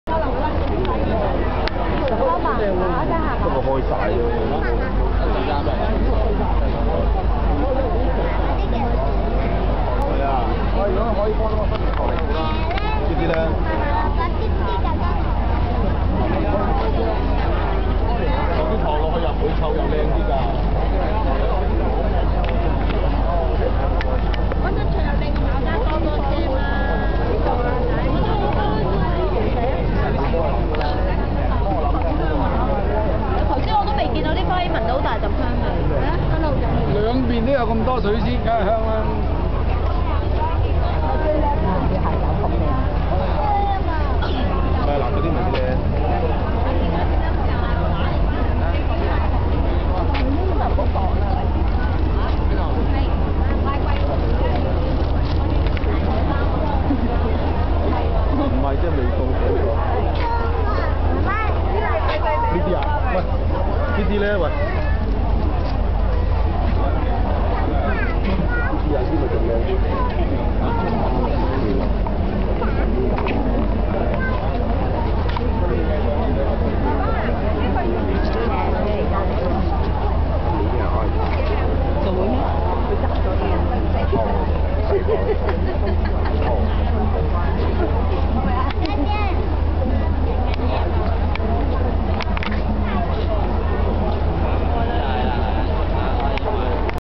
The Lunar New Year Market - Part Two
Among all Chinese festivals, the Lunar New Year is the most important one. It signifies the beginning of a new year. People will sweep the floor, and decorate their houses with flowers and Chinese poems. These bring good luck for the family in the coming year.

Before the new year, people will go to the Lunar New Year market, which is full of people who come for flowers and novelties. There are novelties made especially for this new year and represent the significant like Chief Executive Donald Tsang or the pig of the new year. Big lolly pops, plastic water bottles, Super Mario dolls and flowers such as Peach Blossoms, Narcissus and Mandarin Orange trees are popular during the festival.